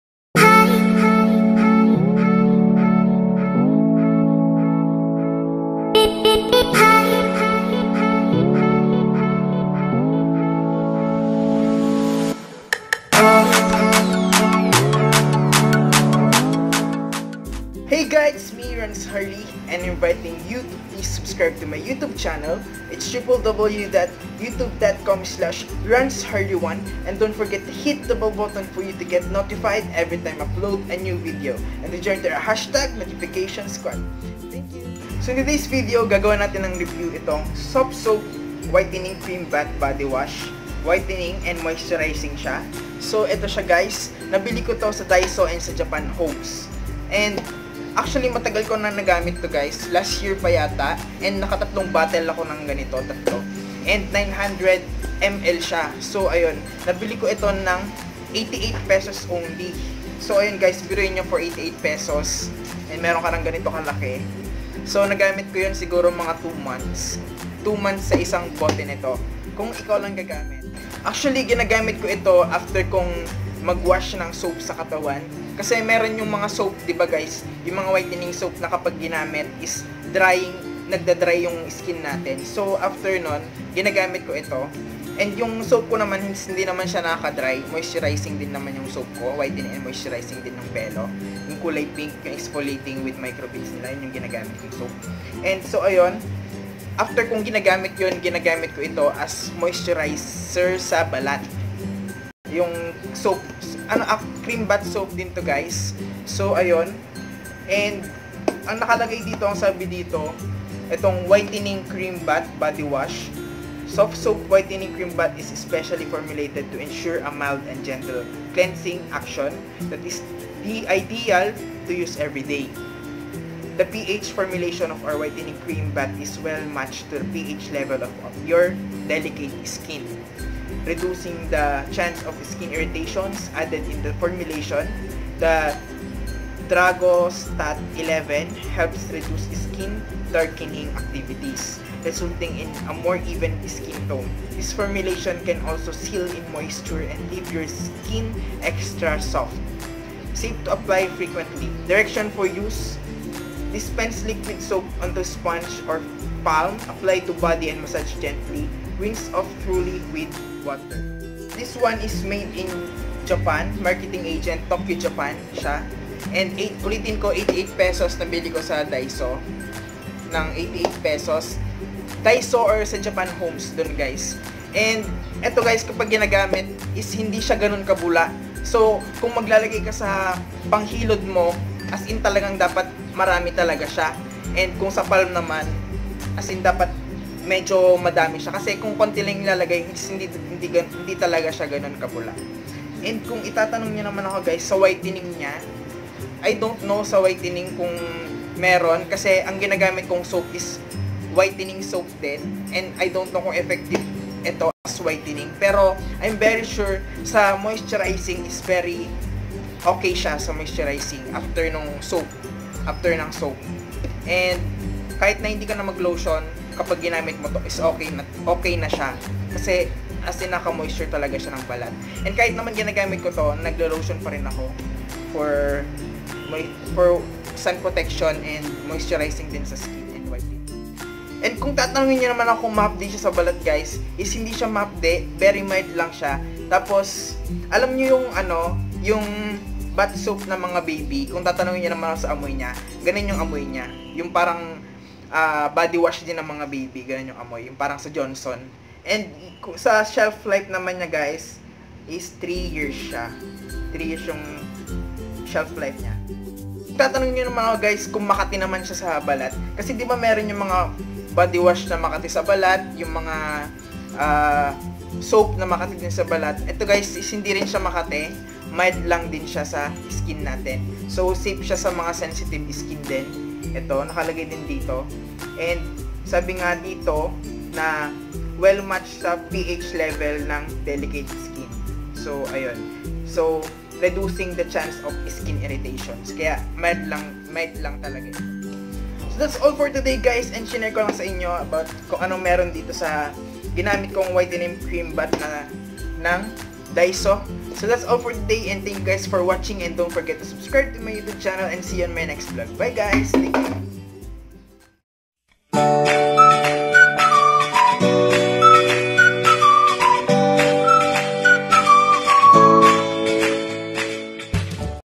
Softsoap Whitening Cream Bath | Review
"Softsoap Whitening Cream Bath" body wash review.

Price:
88 PHP
JOIN TO OUR Harleynatics FAMILY

If you have a product suggestion that you want me to review, comment it down below